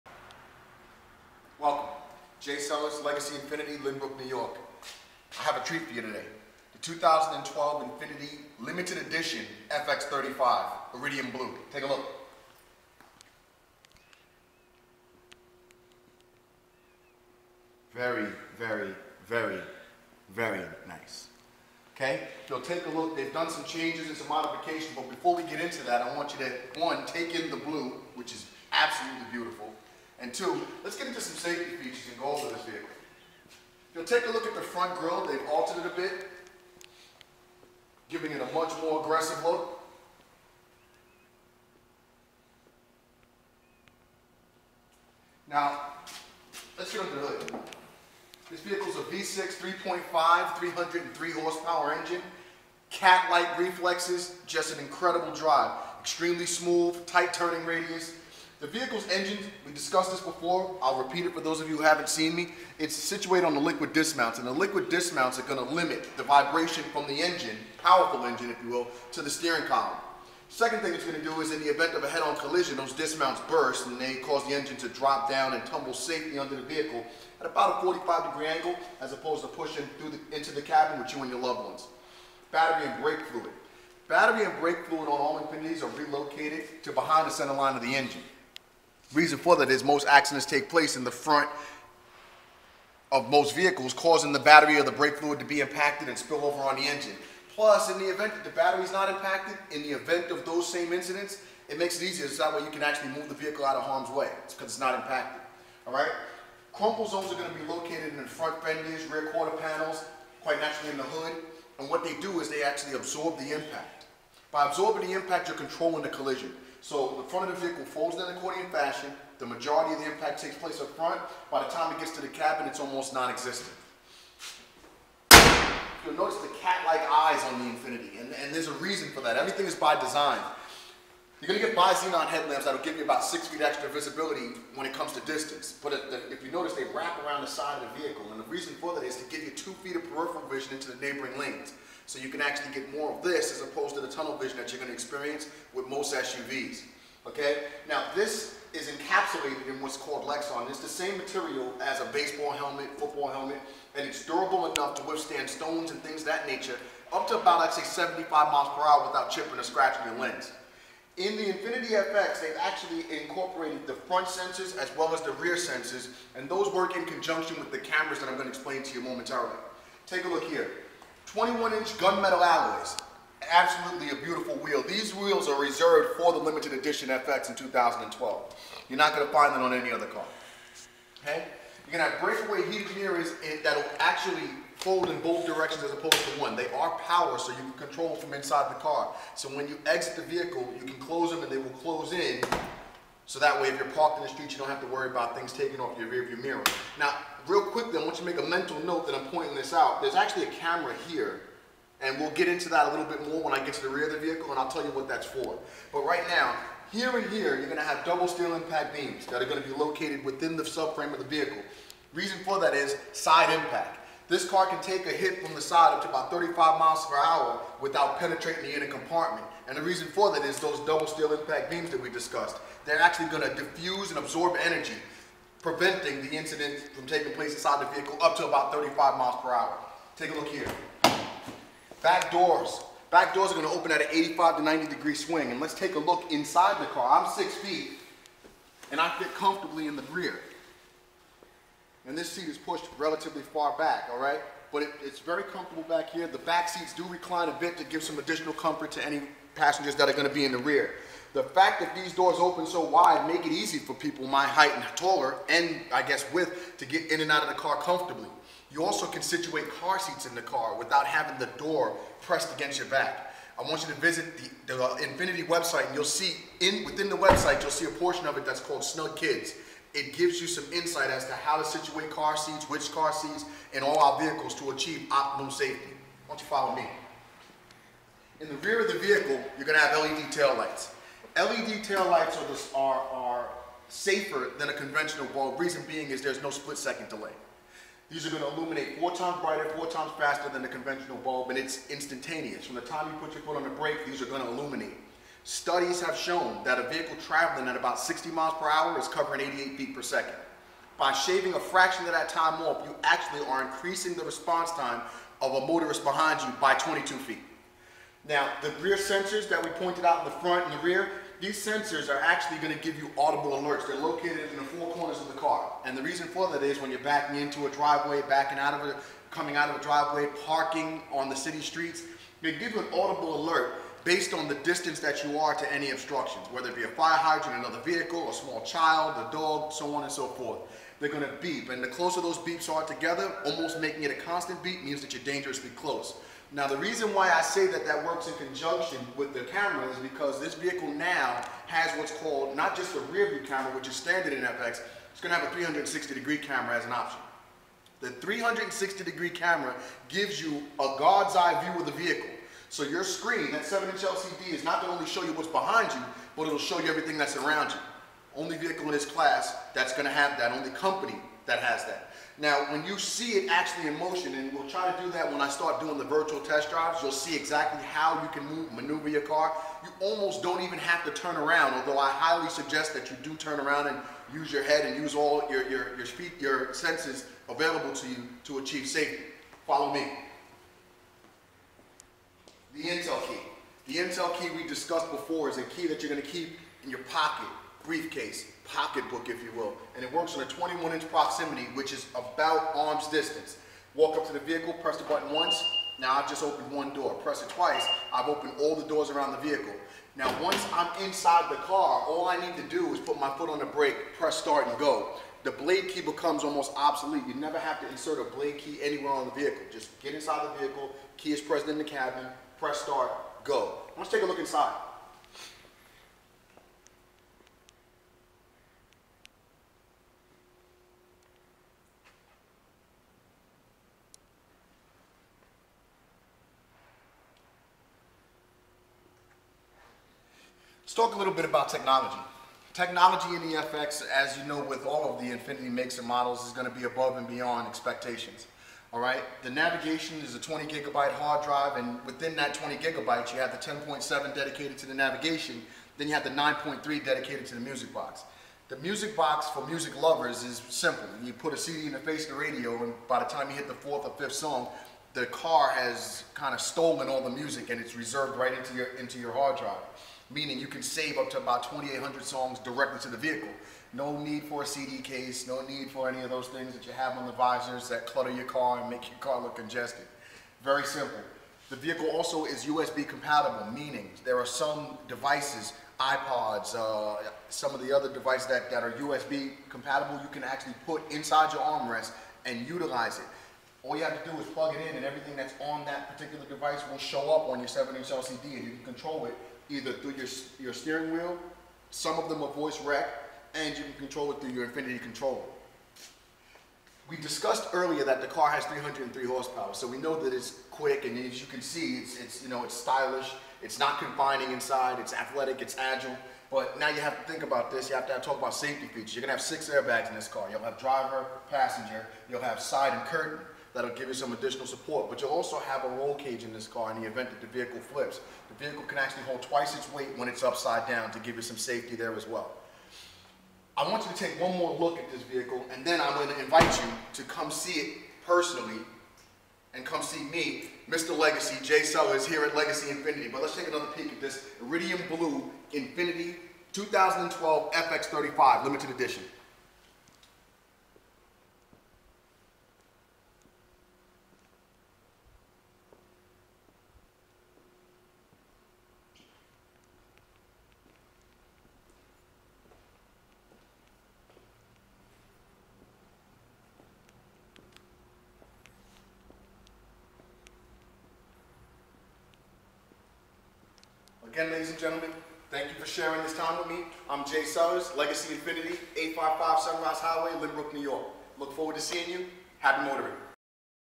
infiniti 2012 FX35 limited edition @ legacy infiniti
Mr. Jay sellers giving a world class walk-around on the 2012 INFINITI FX35 limited edition